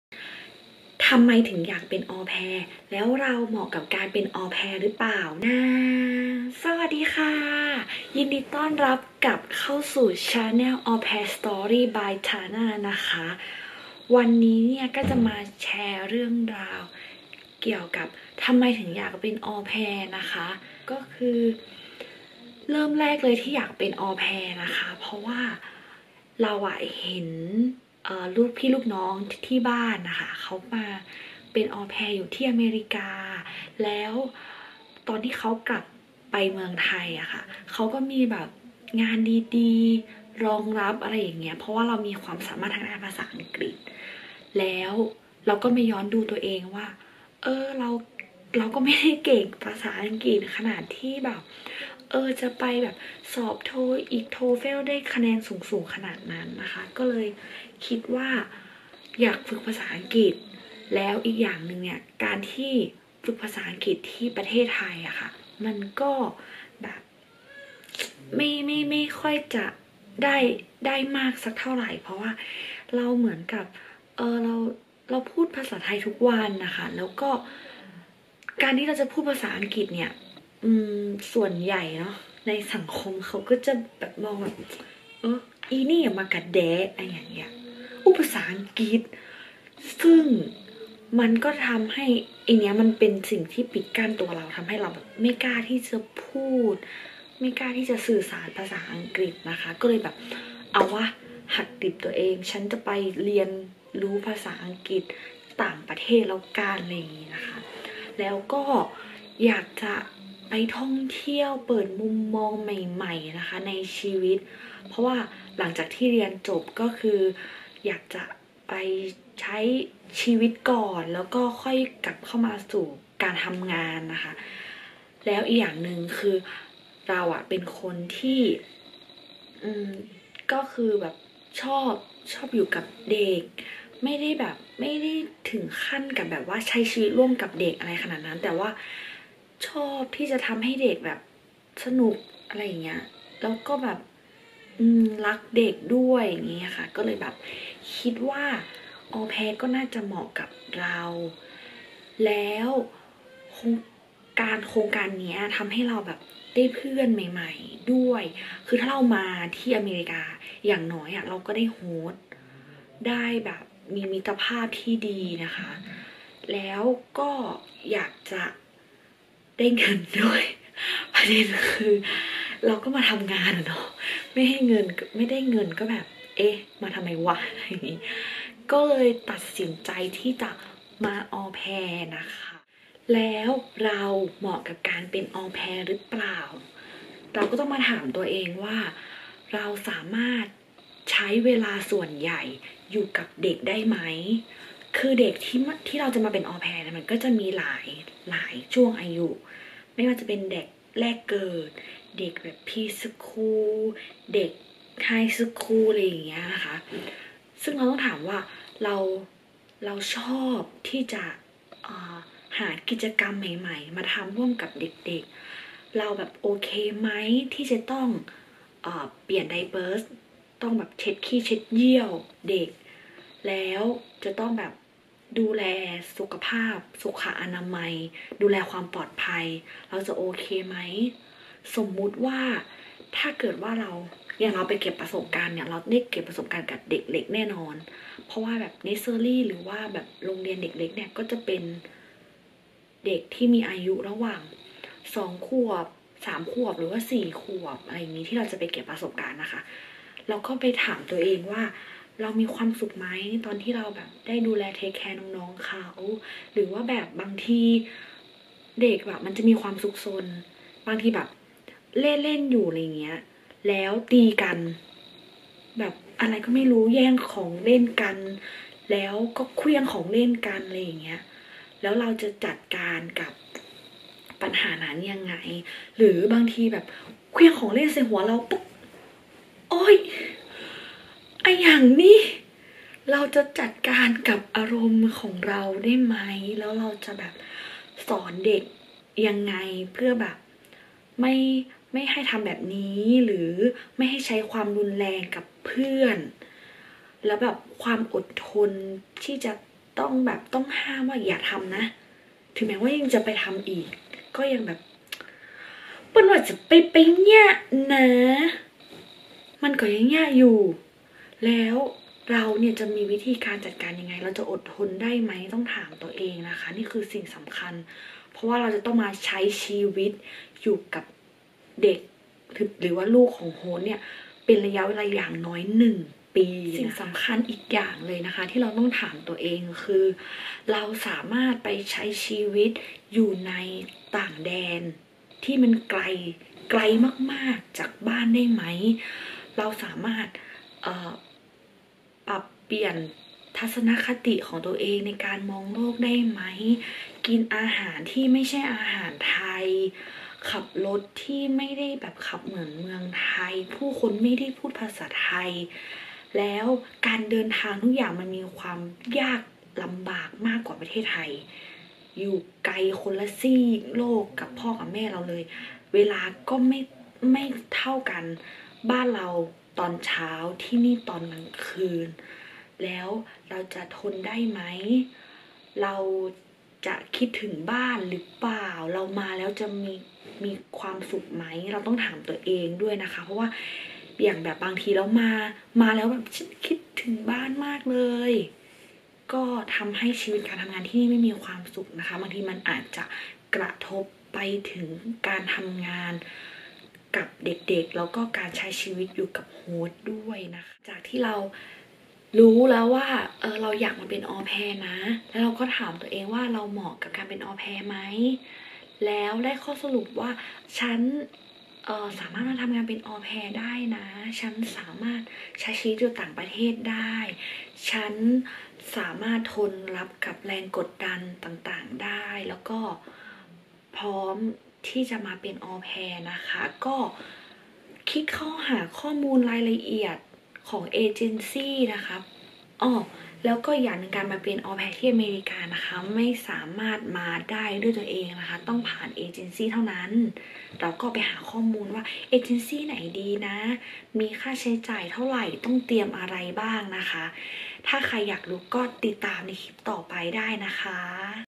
ทำไมถึงอยากเป็นออแพร์ | Aupair in America
คลิปนี้จะแชร์เรื่องราวเกี่ยวกับทำไมถึงอยากเป็นออแพร์ แล้วลักษณะนิสัยของเรา เหมาะกับการเป็นออแพร์หรือเปล่า